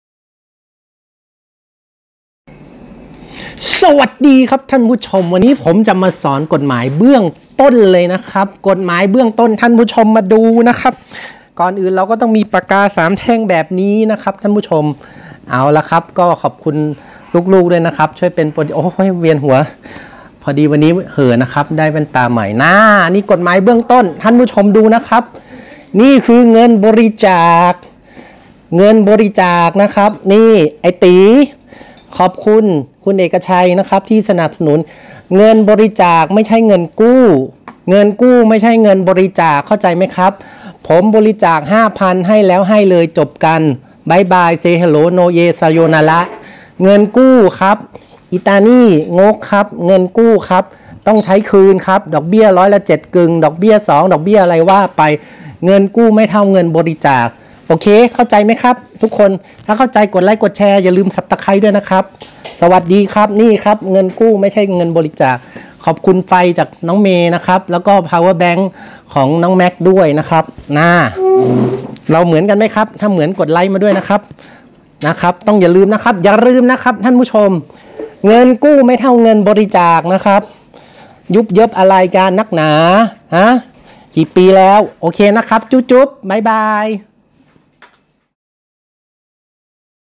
เงินกู้ไม่ใช่เงินบริจาค (พรรคอนาคตใหม่)
นี่ไง ผมบริจาค ๕๐๐๐บาท ให้แล้วให้เลยจบกัน ไม่มีคืน ไม่มีดอกเบี้ย
เงินกู้ คนให้กู้มีภาระทางกฎหมาย ส่งมอบเงินให้ผู้รับ คือคนกู้
ส่วนคนกู้มีหน้าที่ส่งมอบทรัพย์สิน คือเงิน ให้คนให้กู้พร้อมด้วยดอกผลครับ
ง่ายๆแค่นี้ เงินกู้ไม่ใช่เงินบริจาค